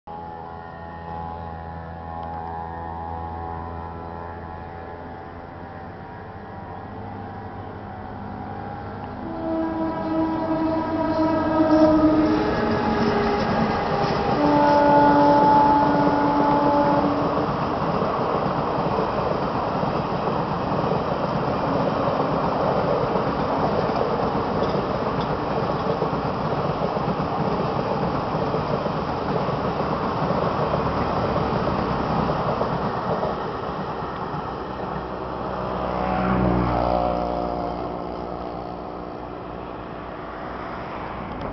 IRFCA - Honking BL WCAM 1 with Surat bound Intercity!
Location: Dahisar Flyover!
BL WCAM 1 '' Pure AC'' with 12935 Bandra T. - Surat Intercity accelerates towards Mira Road!